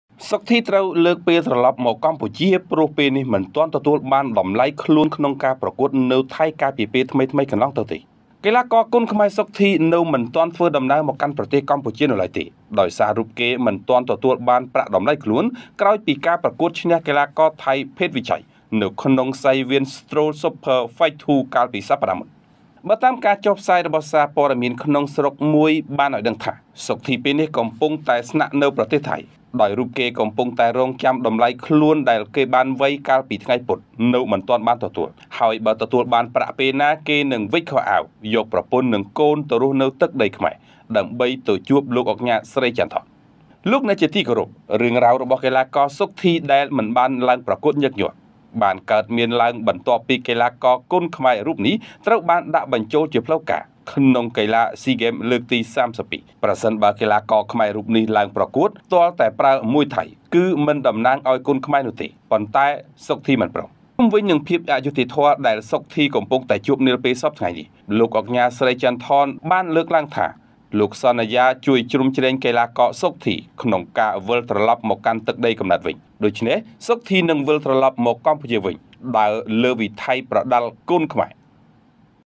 ថៃពិតជាអាក្រក់ណាស់! សុខ ធី មិនទាន់ទទួលបានលុយទេ ក្រោយវៃឈ្នះថៃដោយពន្ទុ
សុខ ធី លើកពេលមកខ្មែរ ក្រោយឈ្នះ សែន ឆៃ ដោយពន្ទុ ហើយមិនបានលុយ...